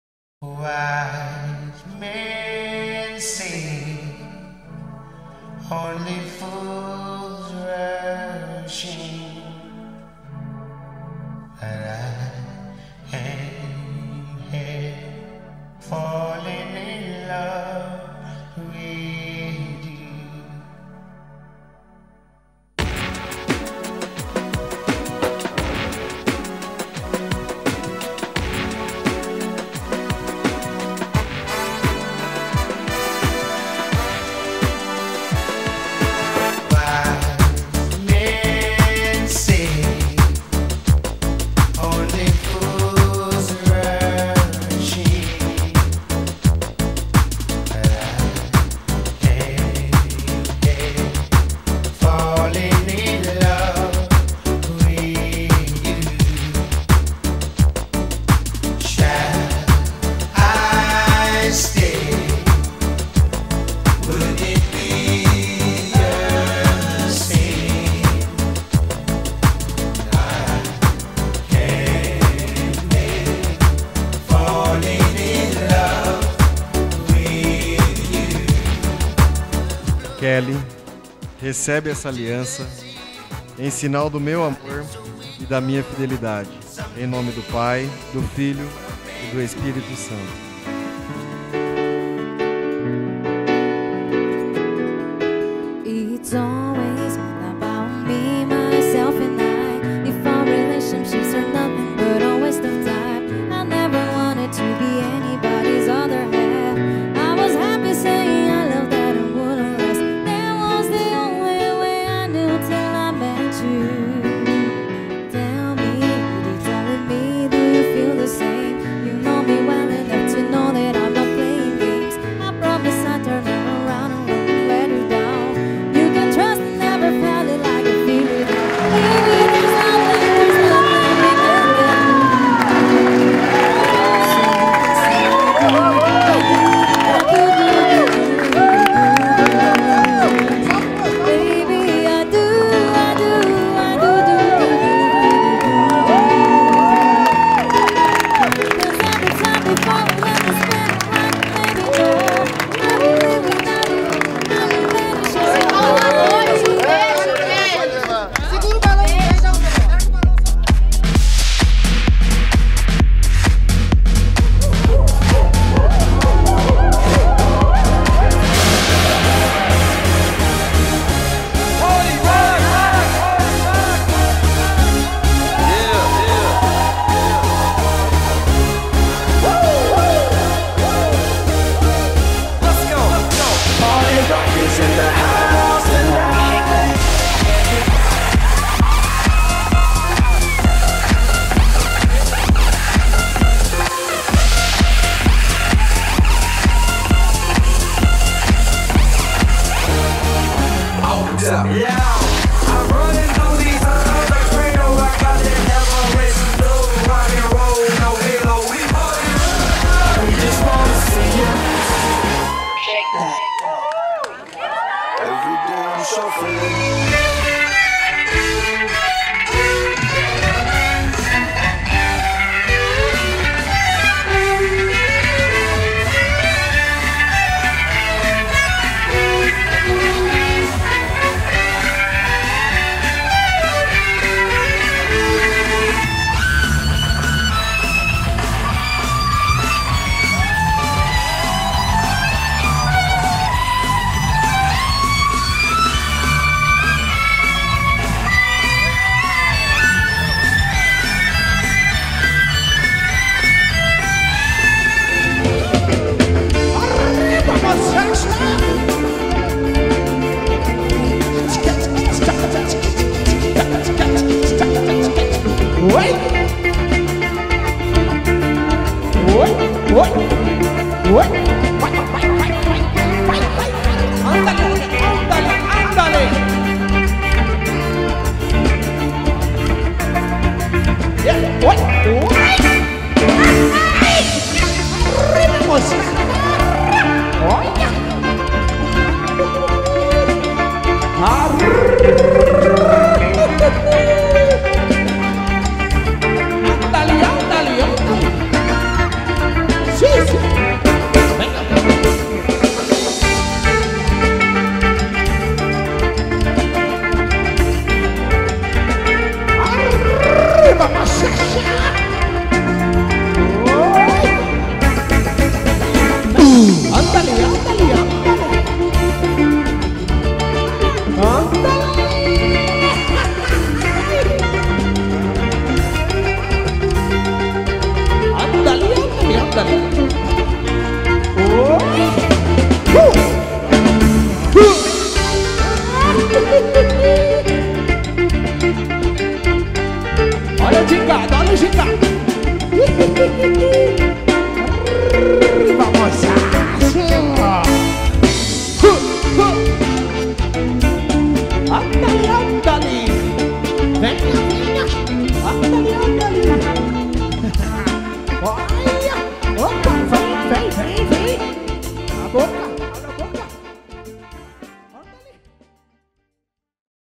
Trailer Keli e Marcos - Josué Pereira Cine Fotografia
O Melhor do casamento de Keli e Marcos, Fotografia e Vídeo por Josué Pereira Cine Fotografia, Decoração: Sirlei Gnann, Keidy Buffet, vestido Sirlei Libanio, doces Celia Feltrin, banda Eros ,Dj Sapinho, cerimonial Erica Gonzales, Cabelo e Maquiagem: SV Cabelereiros, Musical Cerimônia Clara Alves com participação José Junior, Igreja Santa Terezinha do Menino Jesus, celebrante Padre Sandro.